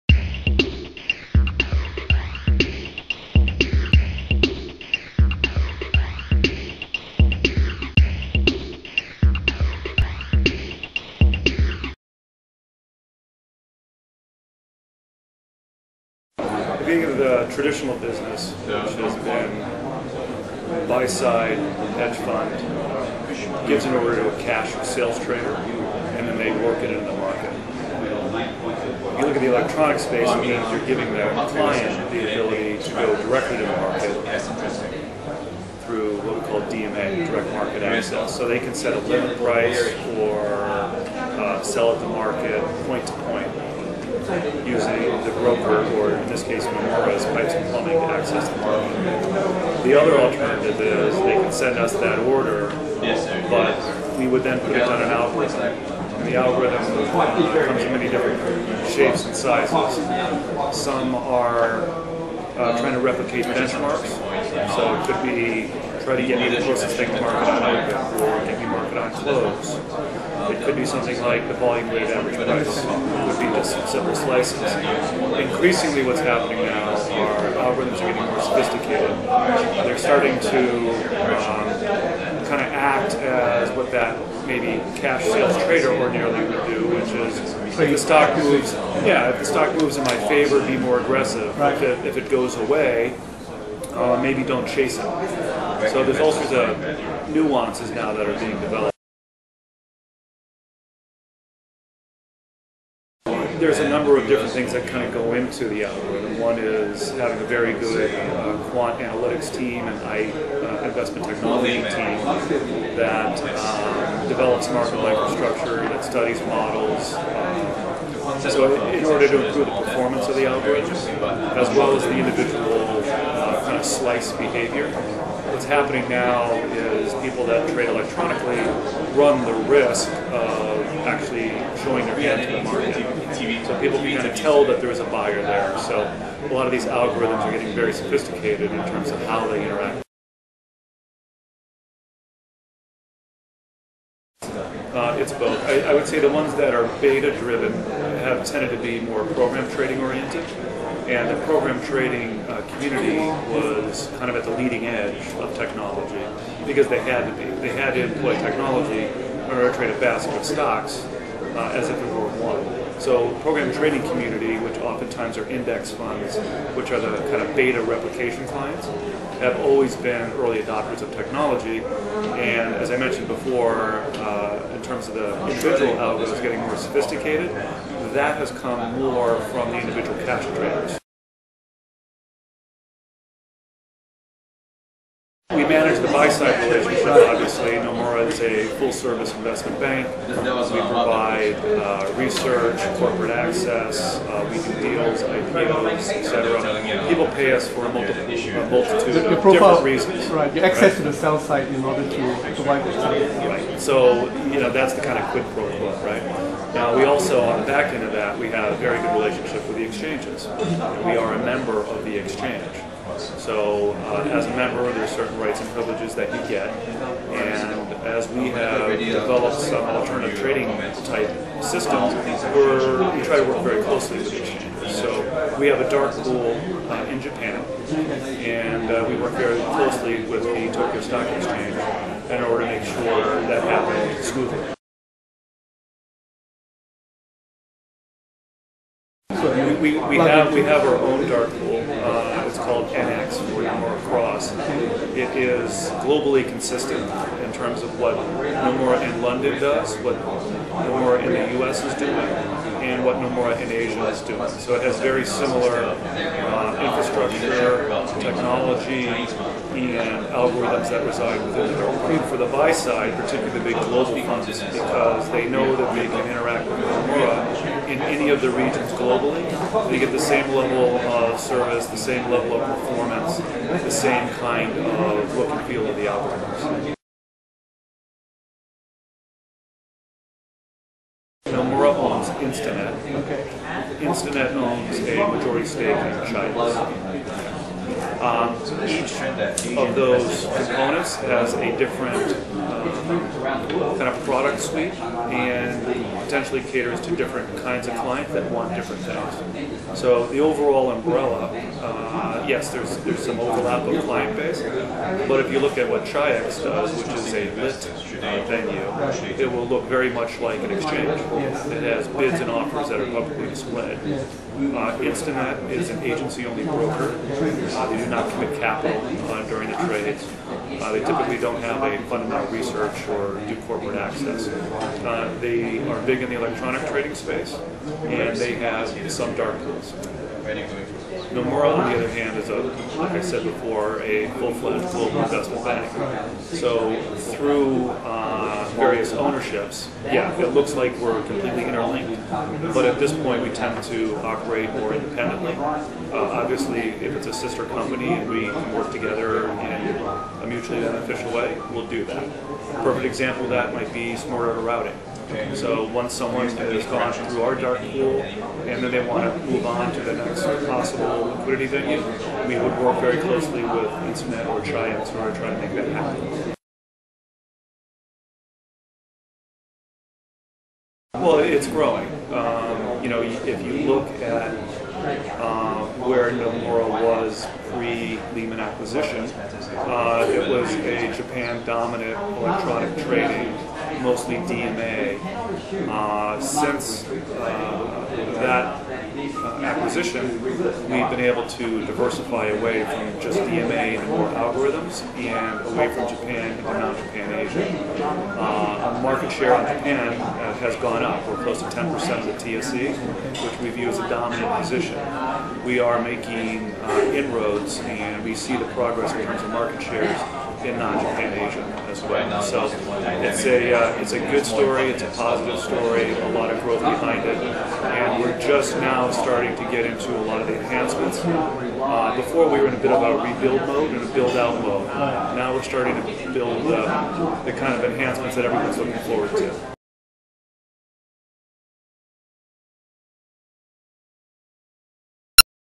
Robert Laible on direct market access and algorithmic trading.
Robert Laible, MD, Electronic Trading AP, Nomura International on direct market access and algorithmic trading.